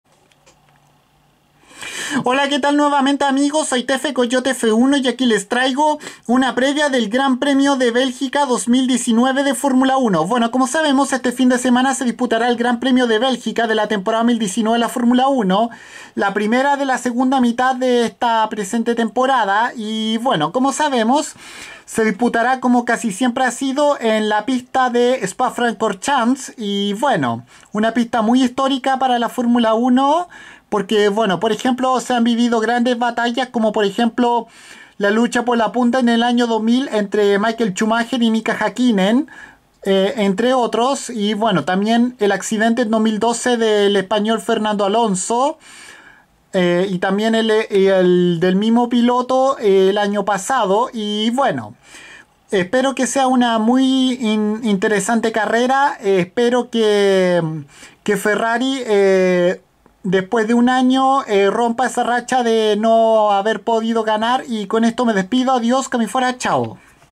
Previa GP de Bélgica 2019 de F1
Una breve previa de la próxima carrera de la Temporada 2019 de la F1 en la que regresan de sus vacaciones de verano en Europa para correr en la famosa pista belga de Spa Francorchamps